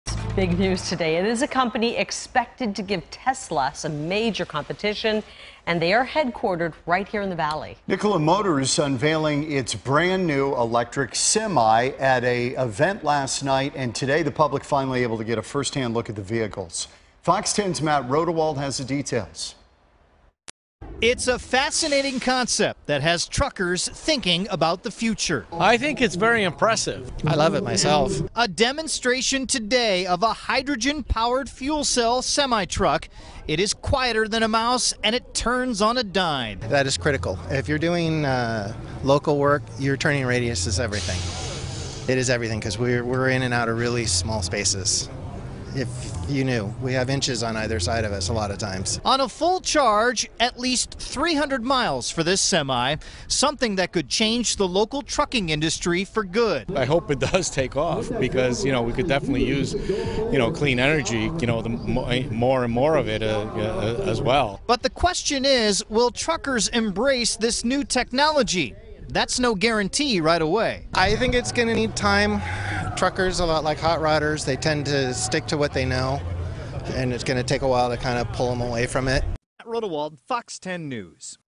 Nikola Motors unveils electric zero emissions semi trucks
A semi trailer that's as quiet as a mouse.